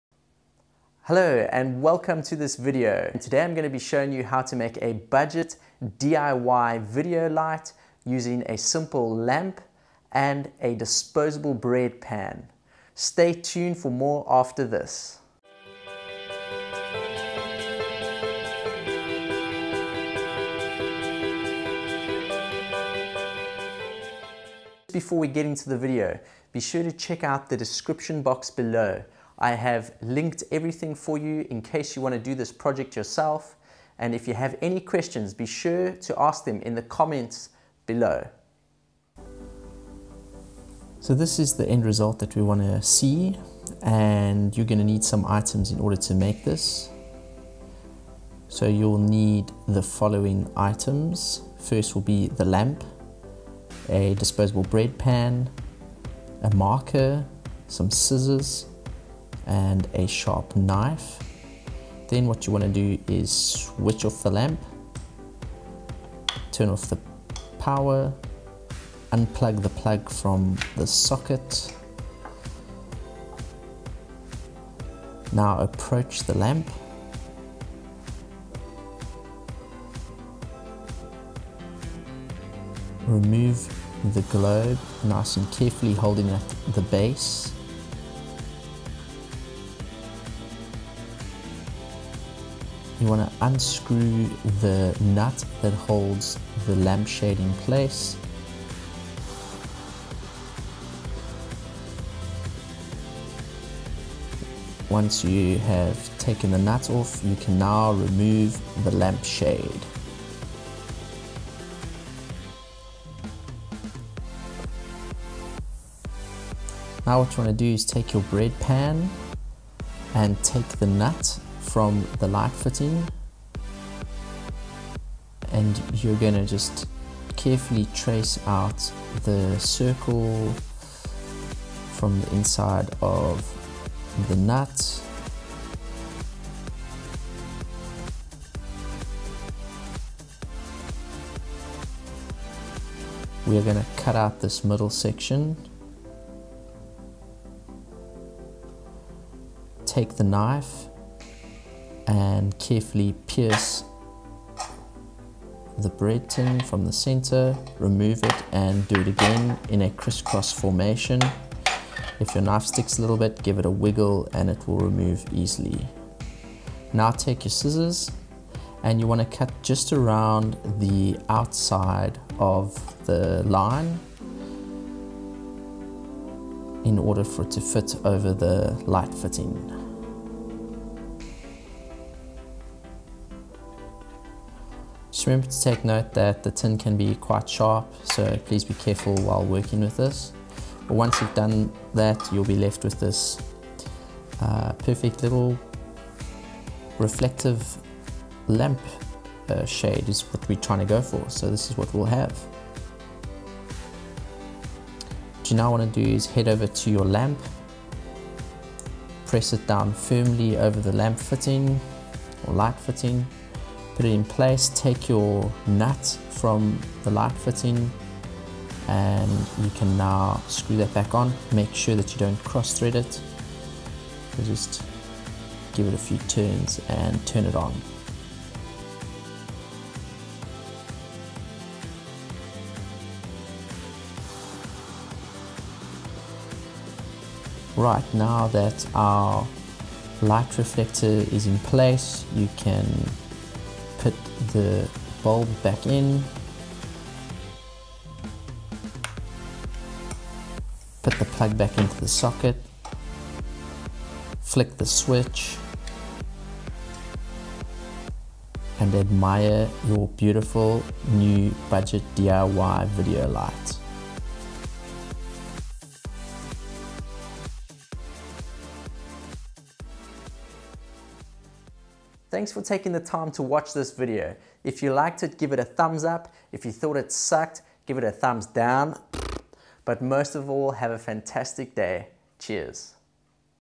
Cheap Video Light
In this video I show you how to make a budget DIY video light

You will need the following items:
Lamp Stand
CFL Bulb
Aluminium Bread Pan

Mobile Video Esstentials:
Mini tripod
Cellular Tripod mount